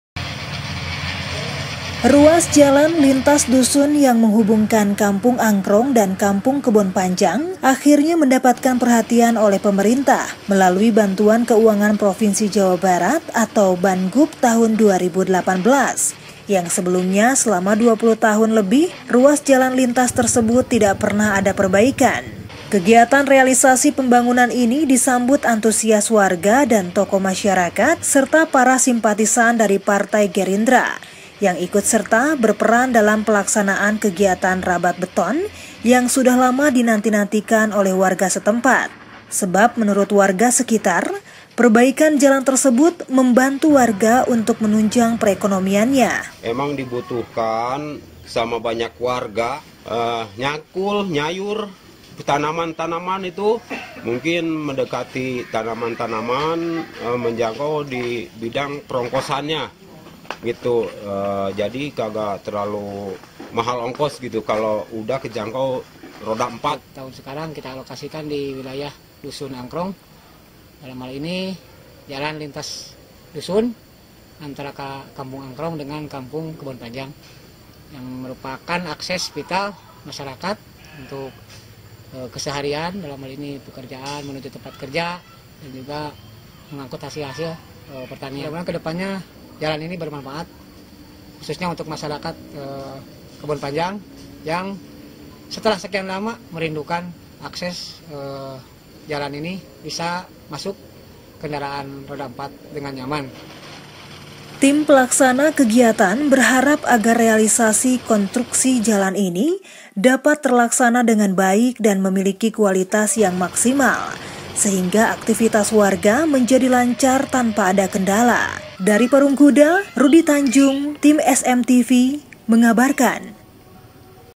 SEPUTAR SUKABUMI - AKHIRNYA , SELAMA 20 TAHUN MENANTI WARGA MERASAKAN AKSES JALAN YANG BAIK LAGI
Lioutan crew SMTV